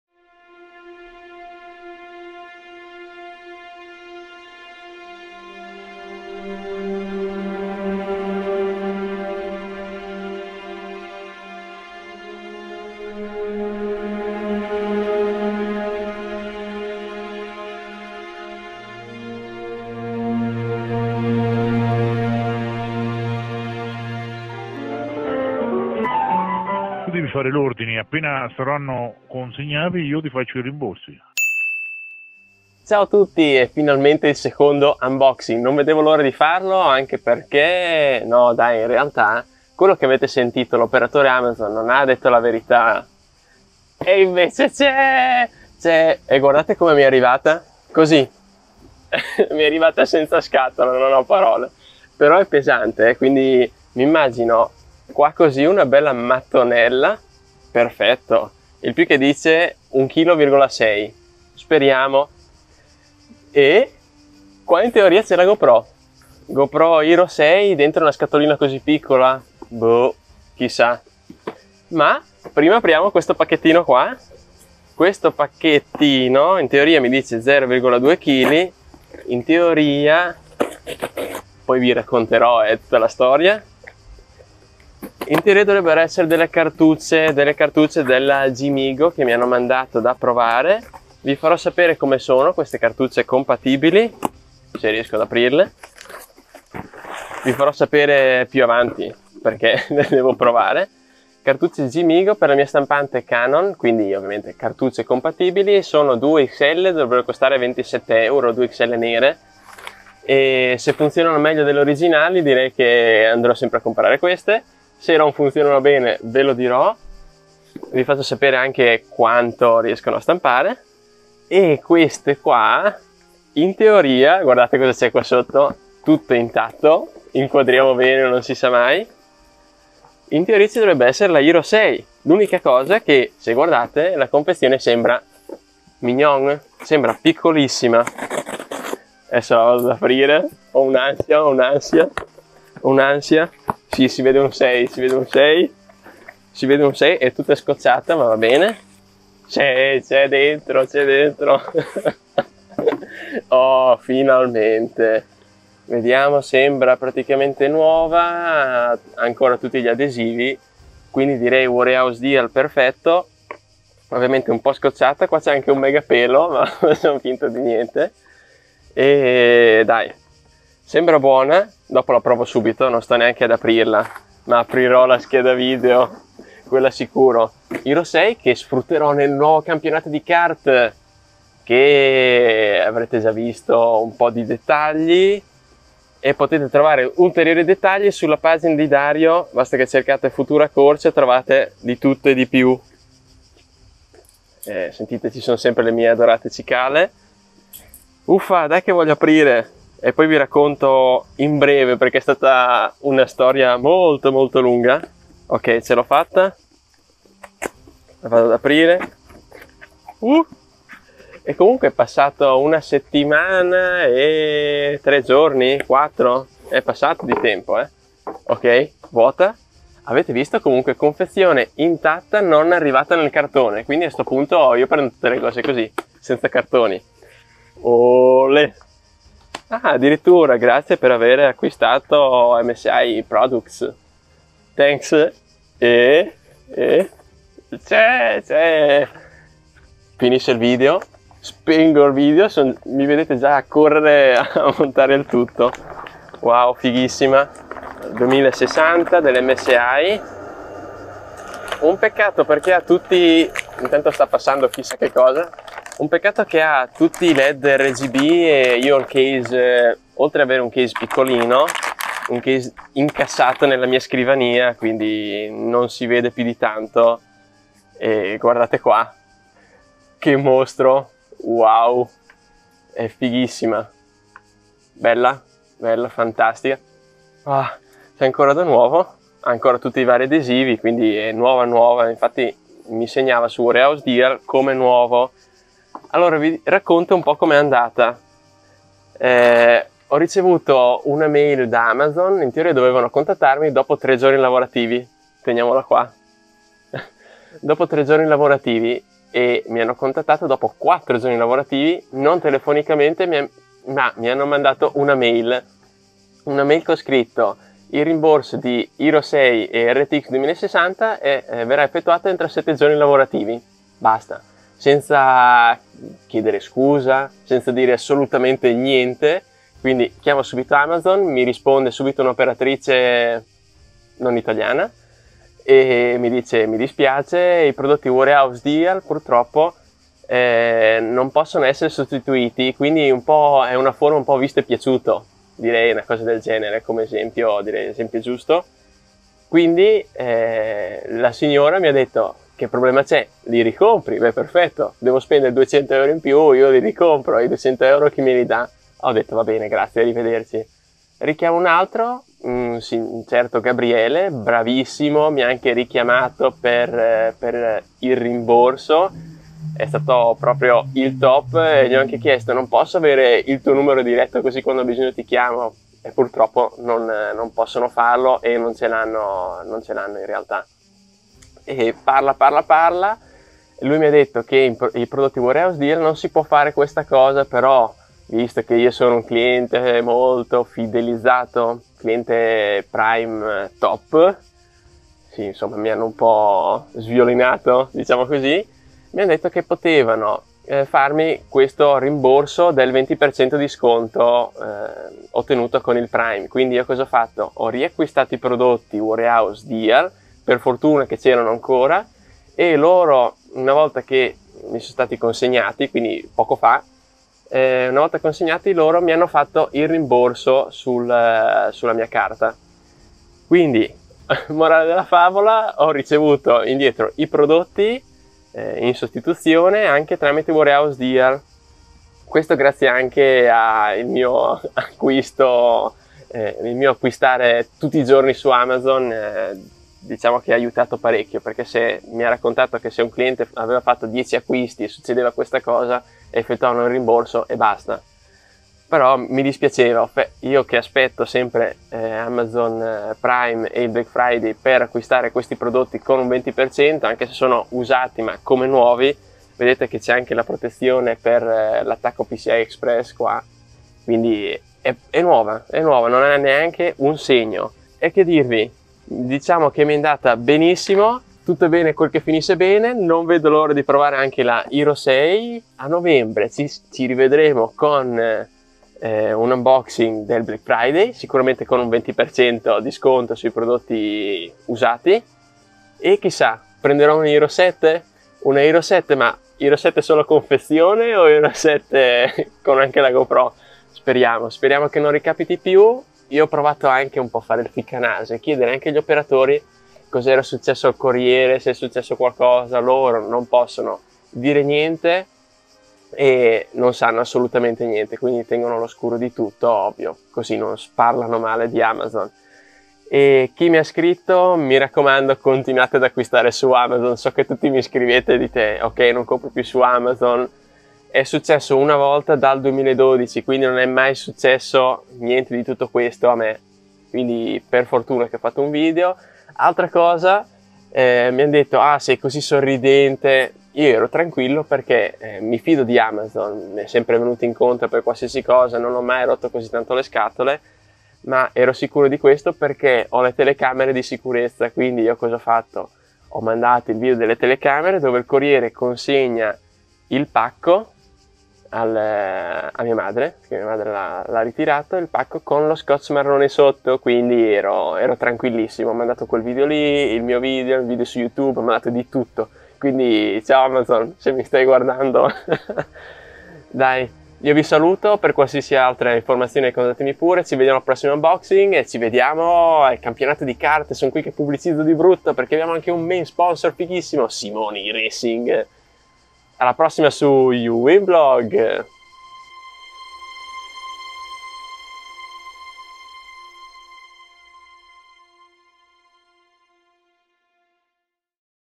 La MIA GoPro e l'RTX 2060? 📦 Unboxing PRIME Day 2019 PT.2
SECONDA Parte dell'unboxing del Prime Day Amazon 2019! 🎁
Vi avevo lasciato con una grande incognita... Ma che fine hanno fatto la mia GoPro e l'MSI RTX 2060? Il corriere si è intascato i miei prodotti? In questo video vi spiego l'accaduto, anche se Amazon non può entrare nei dettagli... 😒
Fatemi sapere se Amazon è stata corretta nei miei confronti e se vi è successo una cosa del genere, scrivetemi tutti i dettagli che sono curioso! 😉👍

◆ PRODOTTI in QUESTO Video PrimeDay 2019
Cartucce JIMIGO 545XL
GoPro Hero 6
MSI GeForce RTX 2060

E non dimenticatevi di iscrivervi al nuovo campionato di Kart OKRS 19/20! Visto che ho la nuova GoPro! 😜